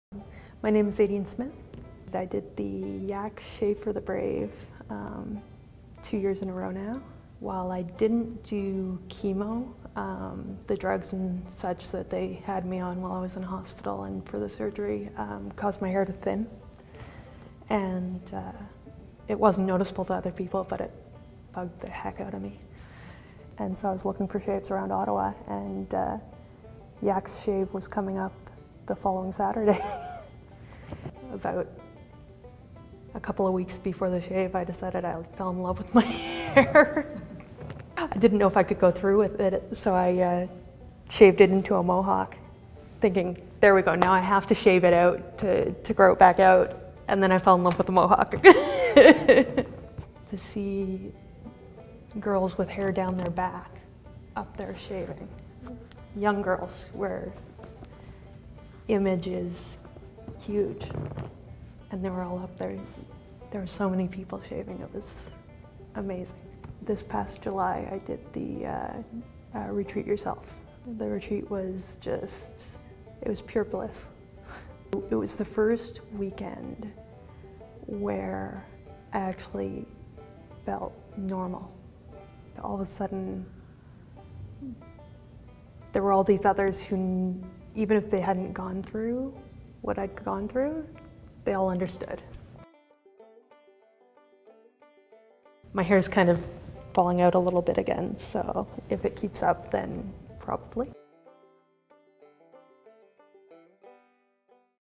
Aideen is Shaving for the third time!
Young adult cancer survivor Aideen Smith is brave. Not only is she a survivor, she has already Shaved twice before, and she was one of the first people to sign up this year!